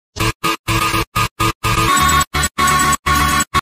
Preview 2 Klasky Csupo Está De Cumpleaños
My new and the first * Preview 2 * I've ever done in my life.

tell me if it is correct or wrong.

Say in the comments if it is the Preview 2 that I made, the following video is about another Preview 2 (Reinvented = Remake)